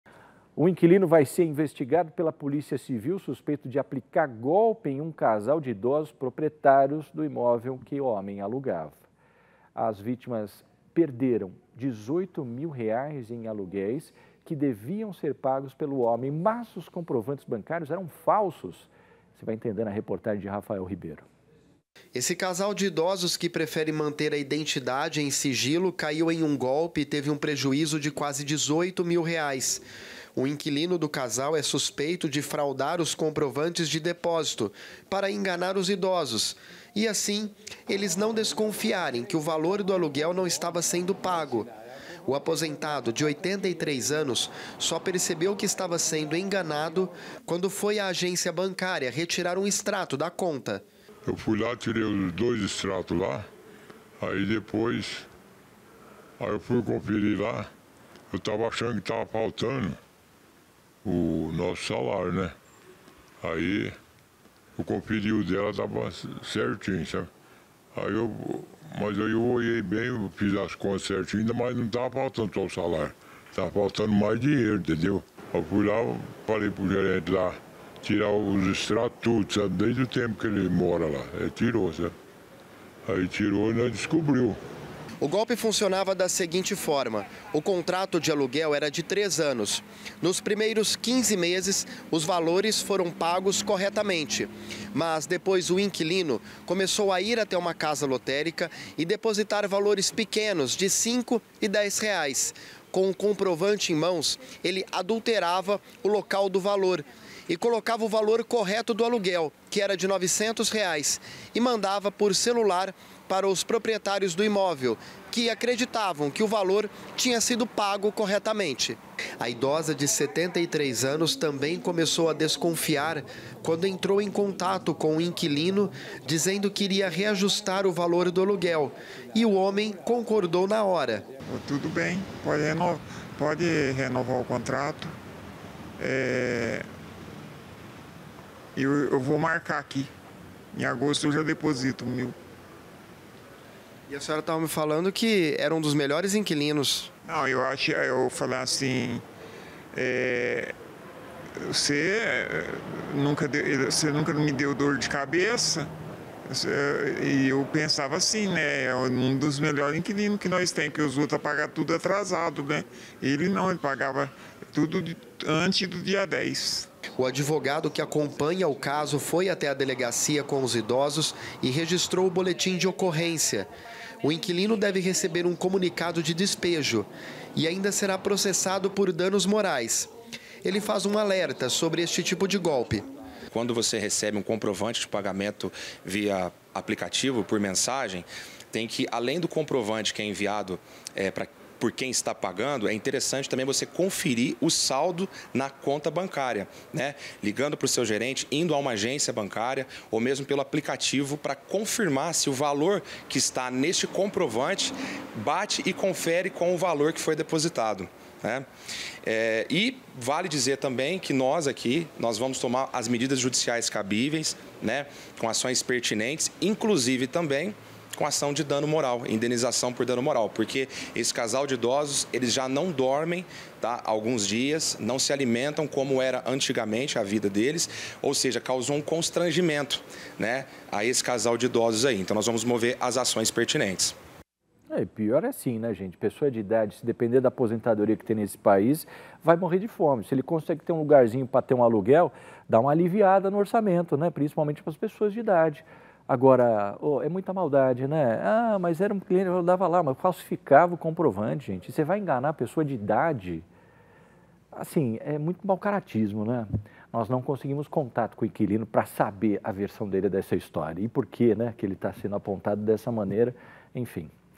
Golpe do aluguel: inquilino mandava comprovantes falsos para idosos donos de imóvel em Franca
Um inquilino vai ser investigado pela Polícia Civil por suspeita de aplicar um golpe em um casal de idosos, proprietários de um imóvel em Franca. O homem enviava comprovantes falsos de depósitos dos aluguéis para as vítimas; o prejuízo é de R$ 18 mil.